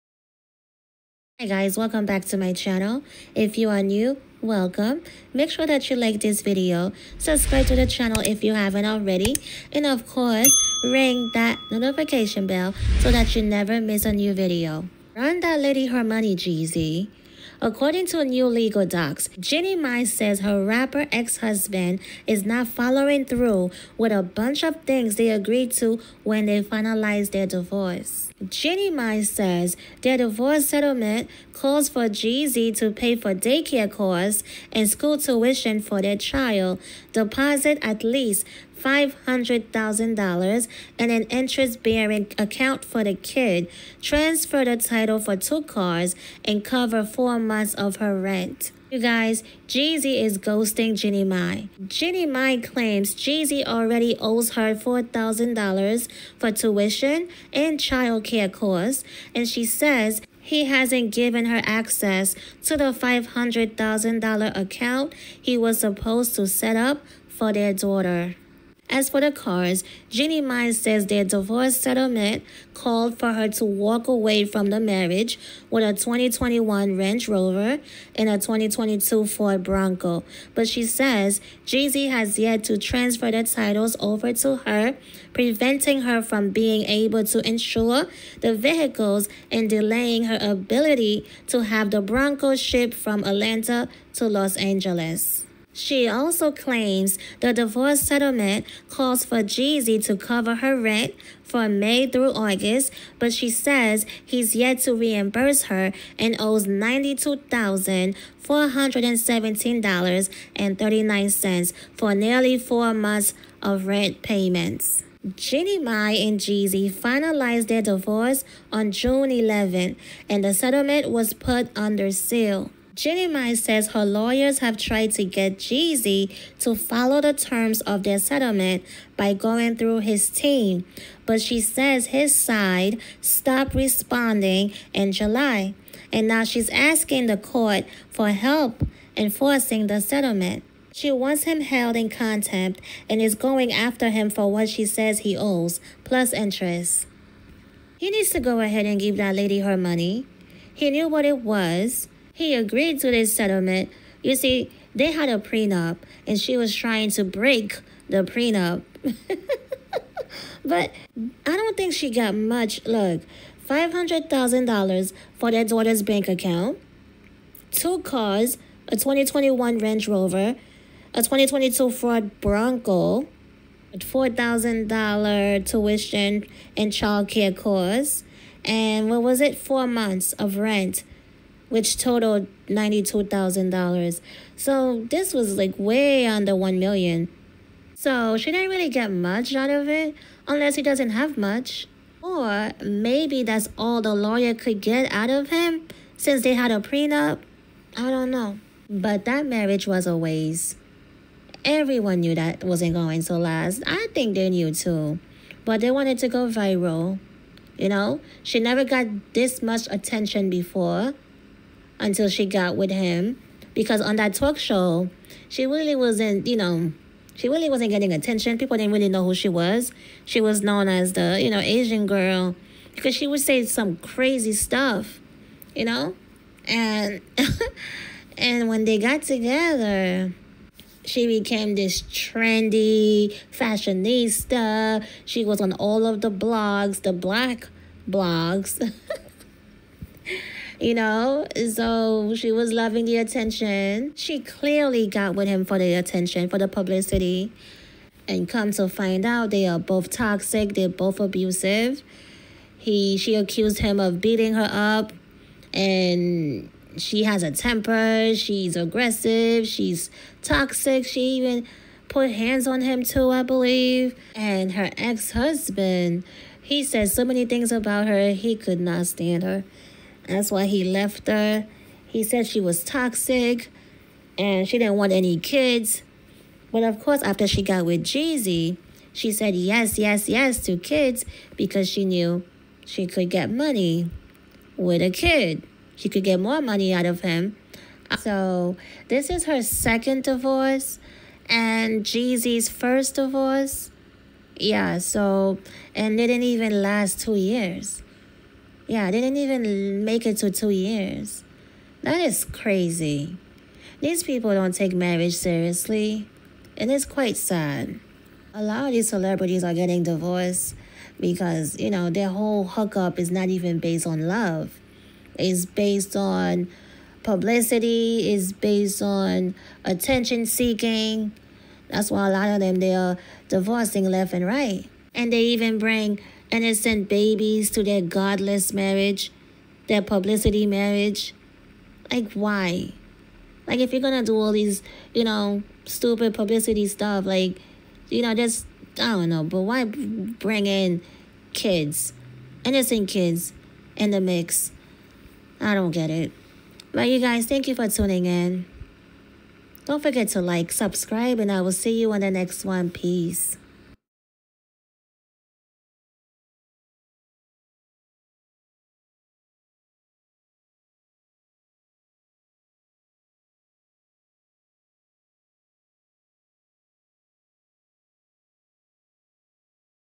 First peek into Jeannie Mai and Jeezy's divorce settlement
Jeannie Mai and Jeezy's divorce settlement was under seal for months but now we get a first peek into their divorce agreement.